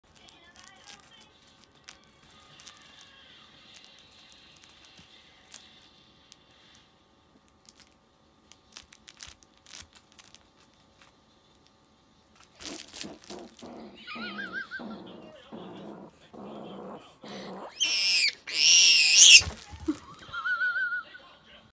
Rabbit having an orgasm FUNNY
Excited rabbit squeals with enjoyment over his toy